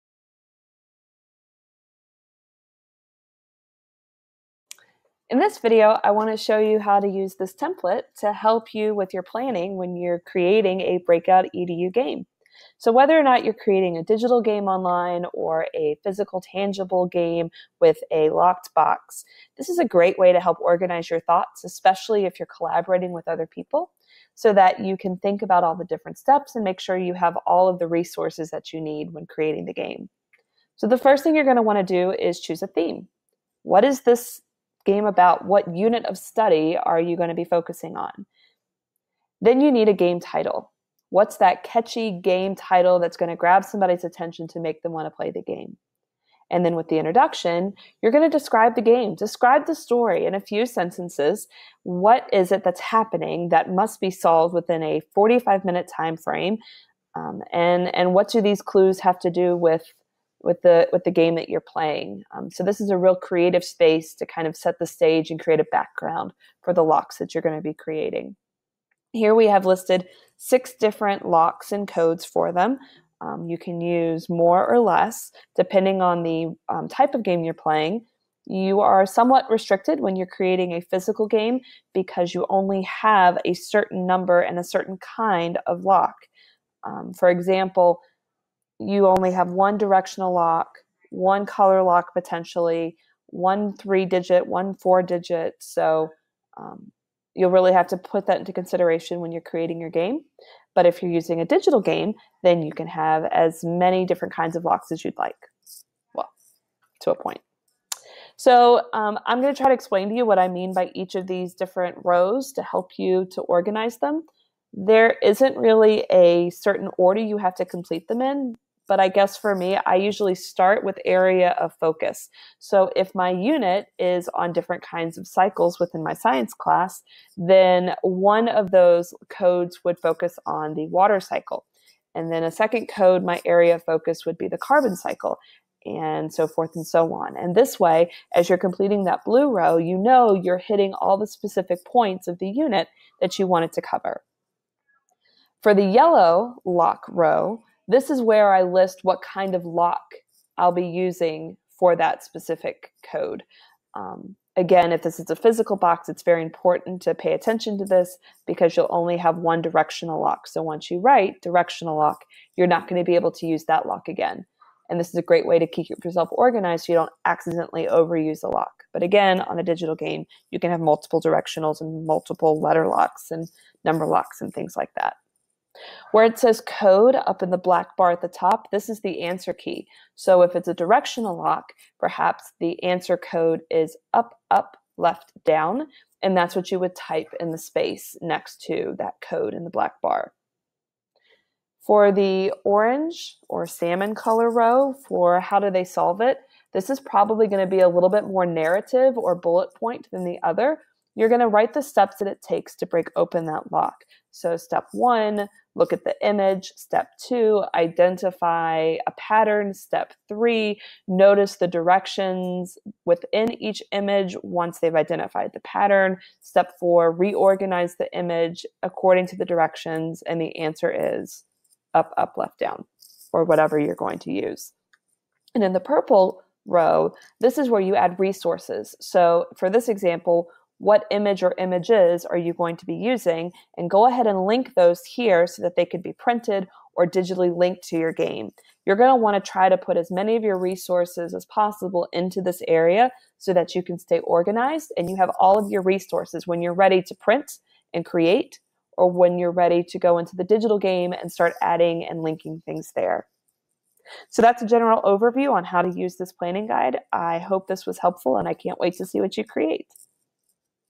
Breakout EDU Planning a digital game
In this video, we'll look at how to fill out a template to help plan your Breakout EDU game.

Click this link to make a copy of your own template